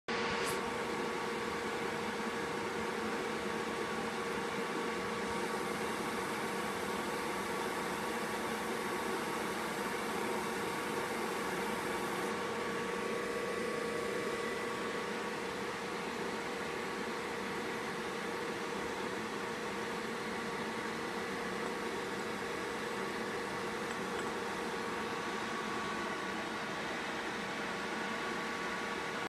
co2 laser marking glass bottle
30w laser marking glass bottle with small control table